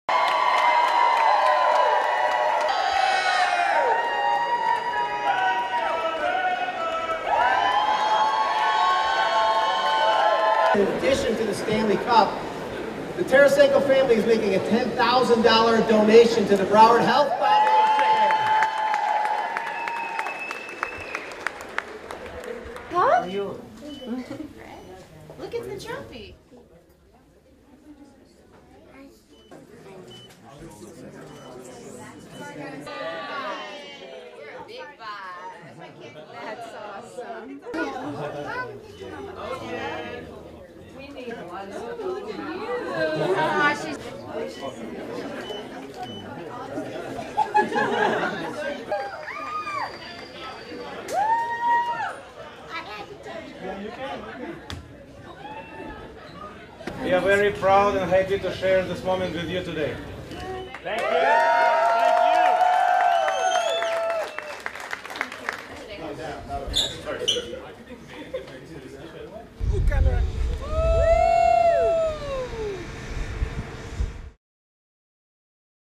Vladimir Tarasenko brings Stanley Cup to Broward Health Medical Center in Florida
Former St. Louis Blues and Florida Panthers player Vladimir Tarasenko brought the Stanley Cup to Broward Health Medical Center to celebrate with families. He also donated $10,000 to the medical center.

Tarasenko won the Stanley Cup with the Panthers. He signed a two-year deal with the Detroit Red Wings.